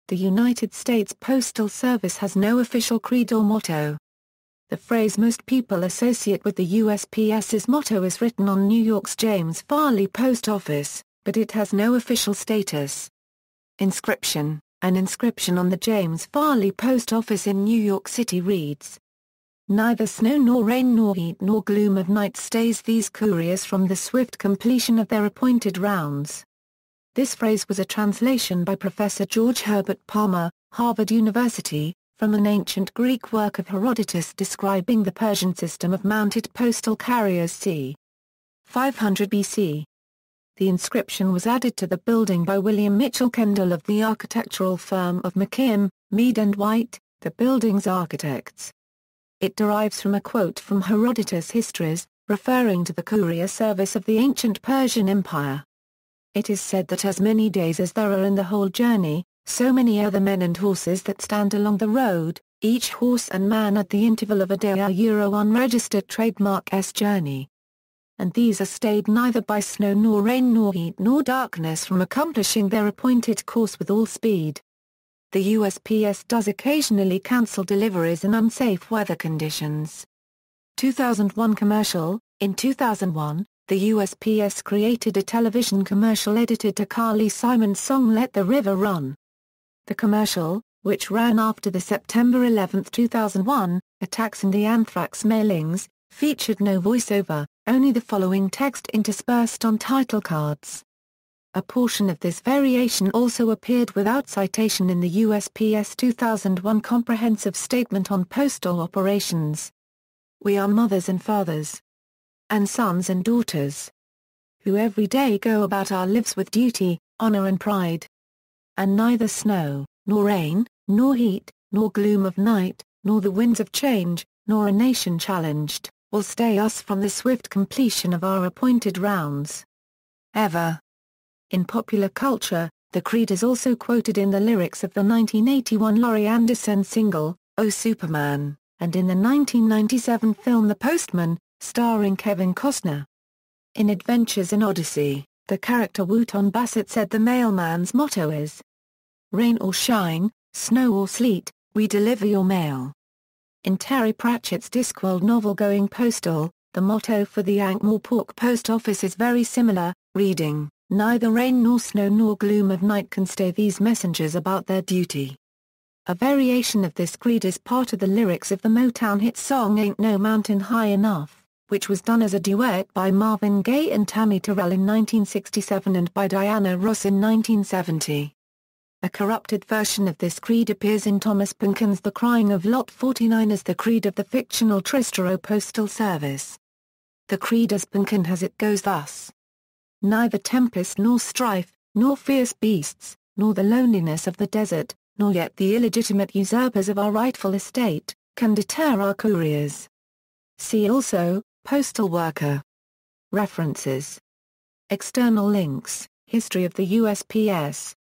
United States Postal Service creed
The United States Postal Service has no official creed or motto. The phrase most people associate with the USPS's motto is written on New York's James Farley Post Office, but it has no official status.